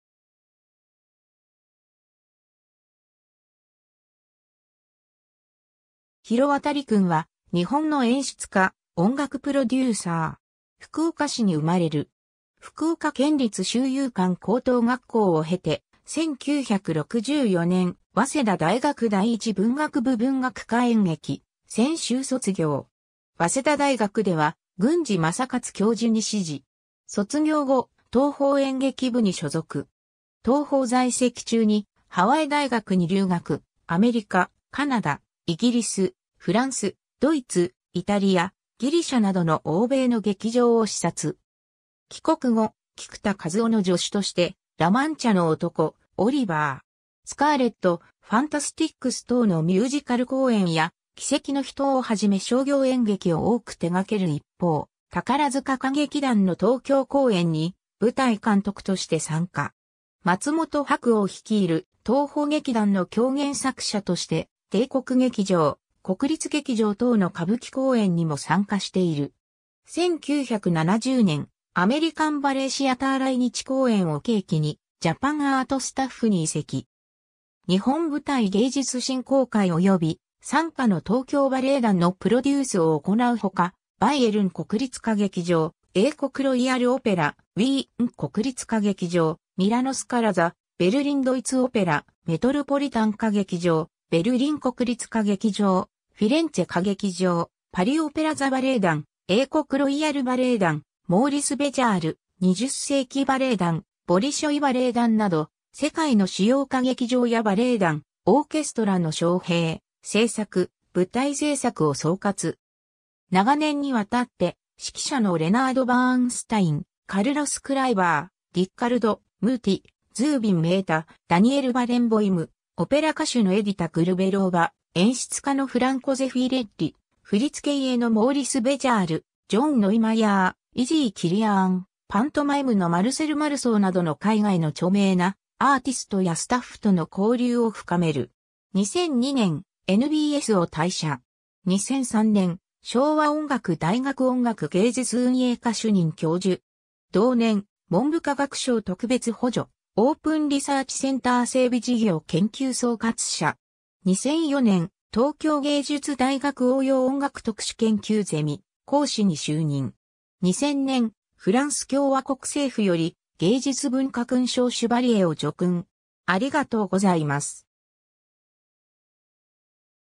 広渡勲
広渡 勲（ひろわたり いさお、1940年12月3日 - ）は日本の演出家、音楽プロデューサー。
 福岡市に生まれる。
福岡県立修猷館高等学校を経て、1964年、早稲田大学第一文学部文学科演劇専修卒業。
早稲田大学では郡司正勝教授に師事。
 卒業後、東宝演劇部に所属。
東宝在籍中にハワイ大学に留学、アメリカ、カナダ、イギリス、フランス、ドイツ、イタリア、ギリシャなどの欧米の劇場を視察。
帰国後、菊田一夫の助手として「ラ・マンチャの男」（日本初演）、「オリバー!」、「スカーレット」、「ファンタスティックス」（日本初演）等のミュージカル公演や、「奇跡の人」（日本初演）をはじめ商業演劇を多く手がける一方、宝塚歌劇団の東京公演に舞台監督として参加。
松本白鸚（現松本幸四郎の父）率いる「東宝劇団」の狂言作者として、帝国劇場、国立劇場等の歌舞伎公演にも参加している。
 1970年、アメリカン・バレエ・シアター来日公演を契機にジャパン・アート・スタッフに移籍。
日本舞台芸術振興会（NBS）及び傘下の東京バレエ団のプロデュースを行うほか、バイエルン国立歌劇場、英国ロイヤル・オペラ、ウィーン国立歌劇場、ミラノ・スカラ座、ベルリン・ドイツ・オペラ、メトロポリタン歌劇場、ベルリン国立歌劇場、フィレンツェ歌劇場、パリ・オペラ座バレエ団、英国ロイヤル・バレエ団、モーリス・ベジャール/20世紀バレエ団、ボリショイ・バレエ団など、世界の主要歌劇場やバレエ団、オーケストラの招聘、制作、舞台製作を総括。
長年にわたって、指揮者のレナード・バーンスタイン、カルロス・クライバー、リッカルド・ムーティ、ズービン・メータ、ダニエル・バレンボイム、オペラ歌手のエディタ・グルベローヴァ、演出家のフランコ・ゼフィレッリ、振付家のモーリス・ベジャール、ジョン・ノイマイヤー、イジー・キリアーン、パントマイムのマルセル・マルソーなどの海外の著名なアーティストやスタッフとの交流を深める。
 2002年、NBSを退社。
2003年、昭和音楽大学音楽芸術運営科主任教授。
同年、文部科学省特別補助オープン・リサーチ・センター整備事業研究総括者。
2004年、東京藝術大学応用音楽特殊研究ゼミ講師に就任。
 2000年、フランス共和国政府より芸術文化勲章シュヴァリエを叙勲。